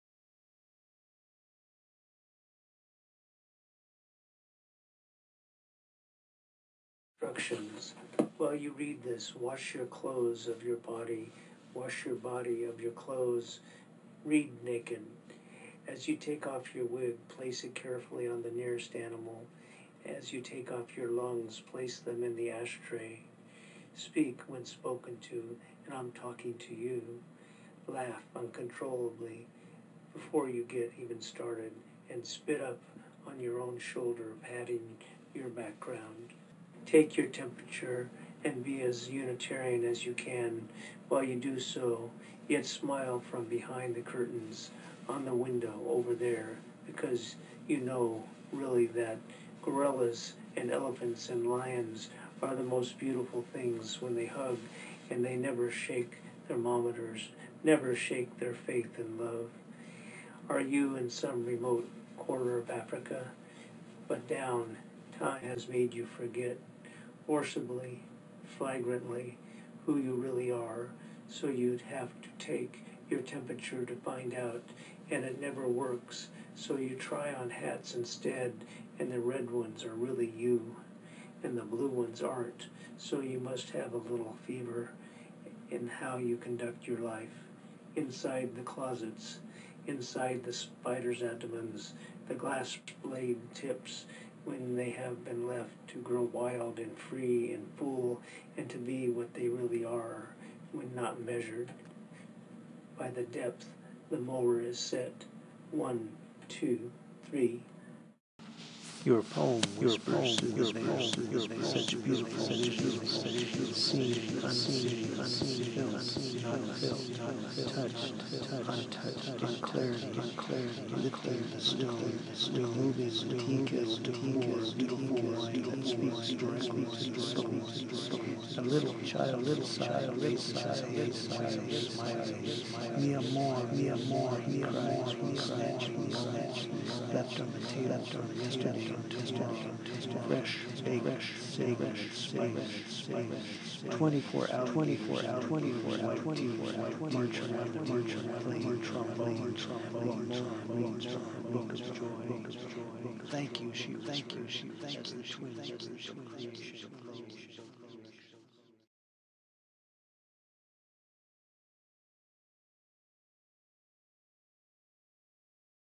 Instructions
Milhouse and Warner recite Bensko beat poetry